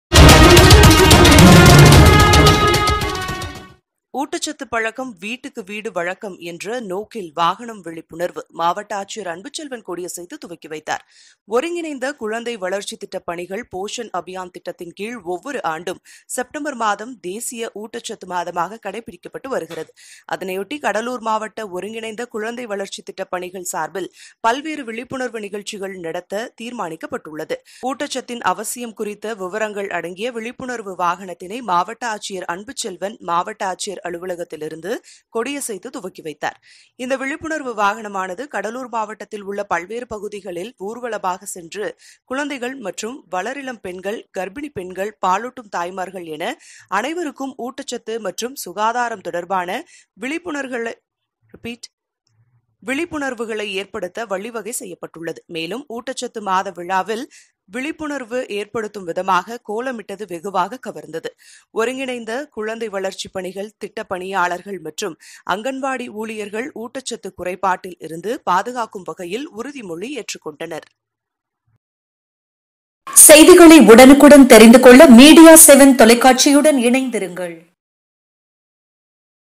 ஊட்டச்சத்து பழக்கம் வீட்டுக்கு வீடு வழக்கம் விழிப்புணர்வு வாகனத்தை -மாவட்ட ஆட்சியர் துவக்கி வைத்தார்
ஊட்டச்சத்து பழக்கம் வீட்டுக்கு வீடு வழக்கம் விழிப்புணர்வு வாகனத்தை -மாவட்ட ஆட்சியர் அன்புச்செல்வன்  கொடியசைத்து துவக்கி வைத்தார்.

ஒருங்கிணைந்த குழந்தை வளர்ச்சித் திட்ட பணிகள் போஷன் அபியான் திட்டத்தின் கீழ் ஒவ்வொரு ஆண்டும் செப்டம்பர் மாதம் தேசிய ஊட்டச்சத்து மாதமாக‌ கடைபிடிக்கப்பட்டு வருகிறது. அதனையொட்டி  கடலூர் மாவட்ட ஒருங்கிணைந்த குழந்தை வளர்ச்சித் திட்டப் பணிகள் சார்பில் பல்வேறு விழிப்புணர்வு நிகழ்ச்சிகள் நடத்த தீர்மானிக்கப்பட்டுள்ளது.
ஊட்டச்சத்தின் அவசியம் குறித்த விவரங்கள் அடங்கிய விழிப்புணர்வு வாகனத்தினை‌ மாவட்ட ஆட்சியர் அன்புச்செல்வன் மாவட்ட ஆட்சியர் அலுவலகத்தில் இருந்து கொடியசைத்து துவக்கி வைத்தார்.இந்த விழிப்புணர்வு வாகனமானது கடலூர் மாவட்டத்தில் உள்ள பல்வேறு பகுதிகளில் ஊர்வலமாக சென்று குழந்தைகள் மற்றும் வளரிளம் பெண்கள் கர்ப்பிணிப் பெண்கள் பாலூட்டும் தாய்மார்கள் என அனைவருக்கும் ஊட்டச்சத்து மற்றும் சுகாதாரம் தொடர்பான விழிப்புணர்வுகளை ஏற்படுத்த வழி வகை செய்யப்பட்டுள்ளது.மேலும் ஊட்டச்சத்து மாத விழாவில் விழிப்புணர்வு ஏற்படுத்தும் விதமாக கோலமிட்டது வெகுவாக கவர்ந்தது.ஒருங்கிணைந்த குழந்தை வளர்ச்சிப் பணிகள் திட்ட பணியாளர்கள் மற்றும் அங்கன்வாடி ஊழியர்கள் ஊட்டச்சத்து குறைபாட்டில் இருந்து பாதுகாக்கும் வகையில் உறுதிமொழி ஏற்றுக்கொண்டனர்.